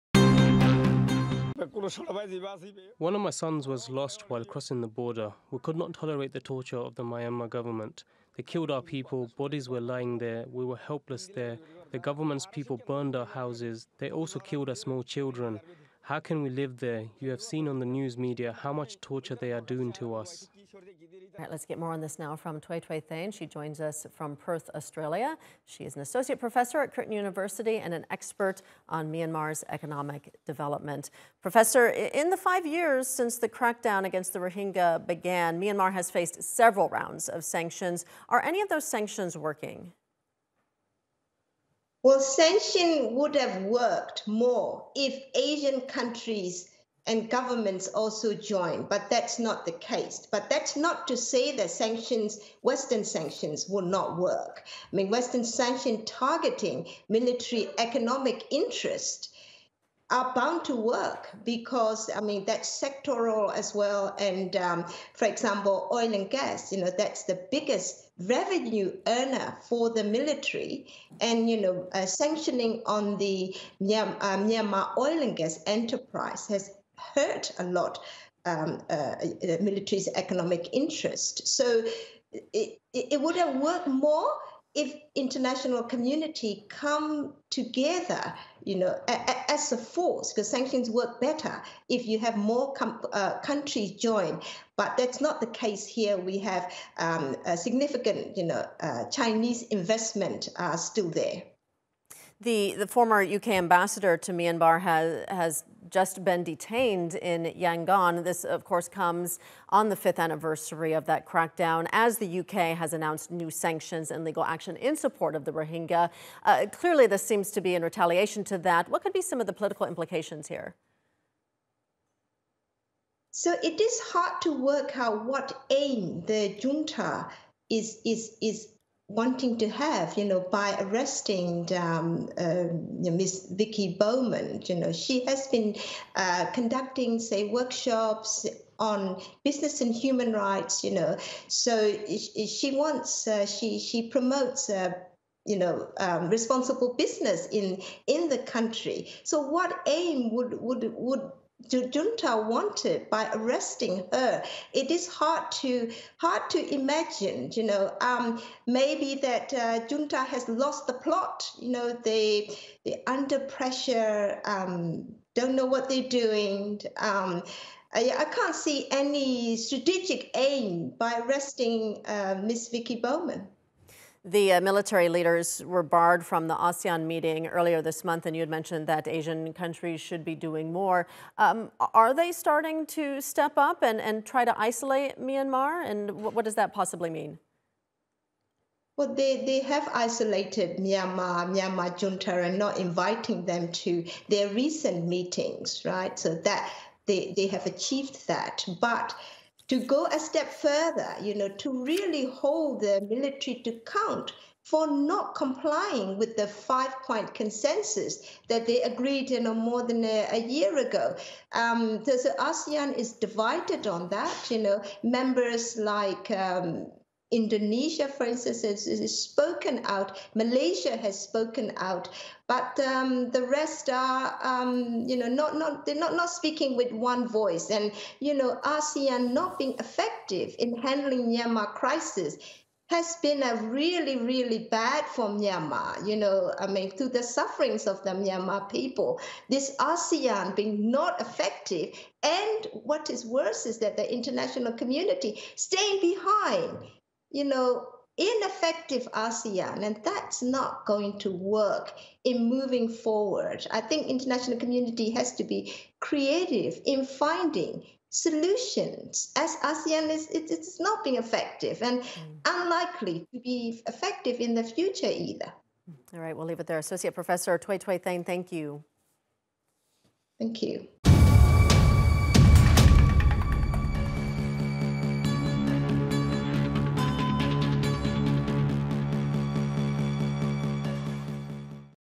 Rohingya mark five years since exodus to Bangladesh
It's been five years since Myanmar government troops began a violent crackdown on the Rohingya Muslim community. It sparked a huge exodus of refugees and charges of genocide against the Myanmar regime. TRT World speaks to Htwe Htwe Thein from Curtin University on whether sanctions against the military junta has achieved anything.